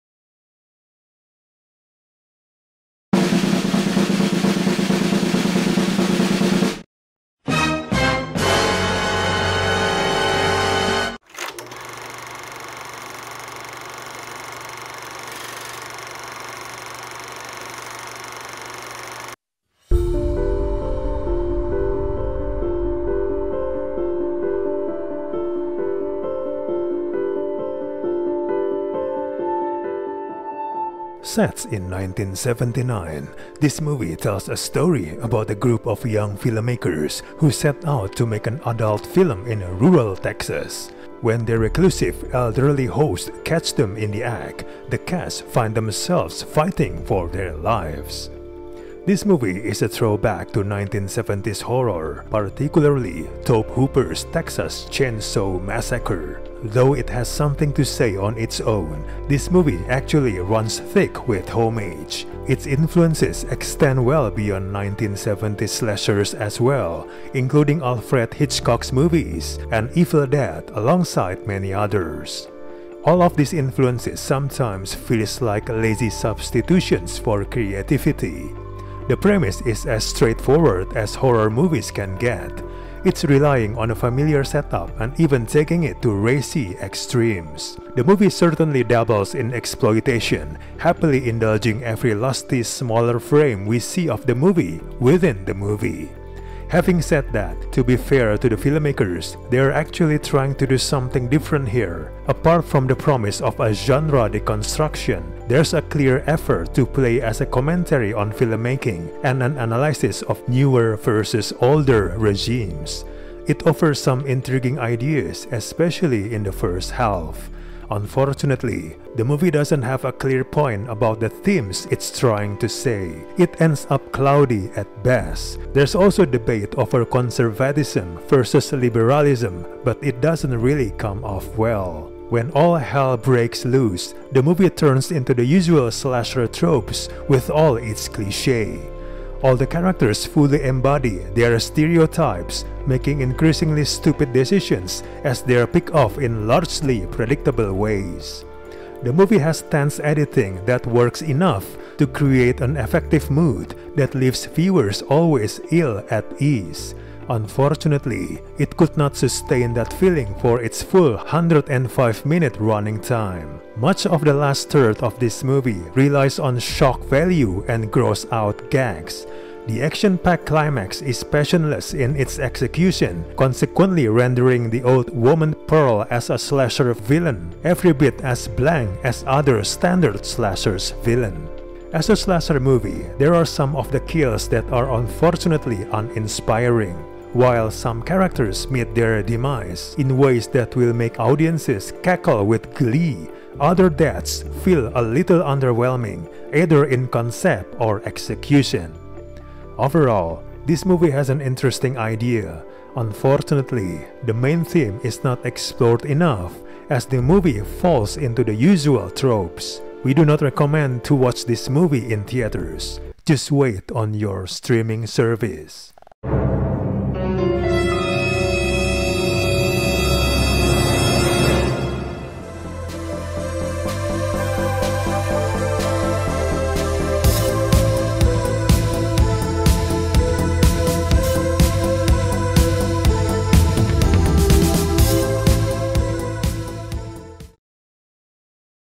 X Horror Review - Bad Movie Reviews
In 1979, a group of young filmmakers set out to make an adult film in rural Texas, but when their reclusive, elderly hosts catch them in the act, the cast find themselves fighting for their lives.

Directed by : Ti West

Starring :

- Mia Goth
- Jenna Ortega
- Brittany Snow
- Kid Cudi
- Martin Henderson
- Owen Campbell
- Stephen Ure
- James Gaylyn
- Simon Prast
- Geoff Dolan
- Matthew J. Saville
- Bryony Skillington